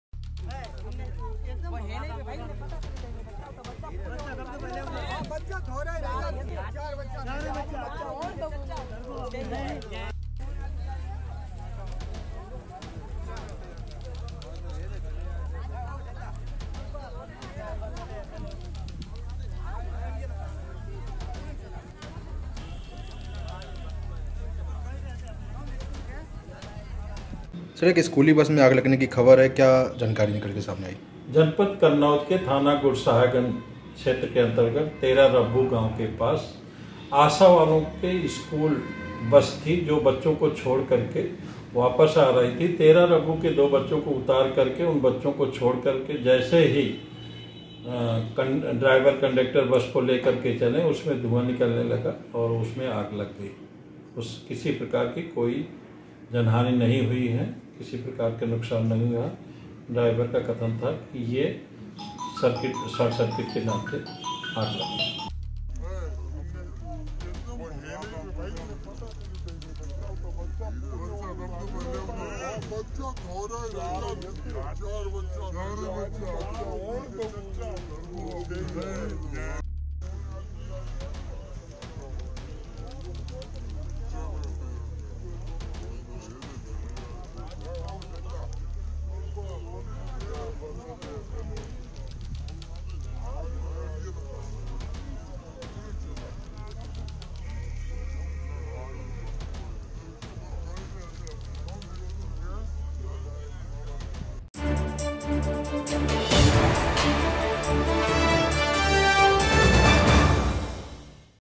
विद्यालय जा रही स्कूल बस बनी आग का गोला | School Bus | Fire | Kannauj News
विद्यालय जा रही स्कूल बस बनी आग का गोला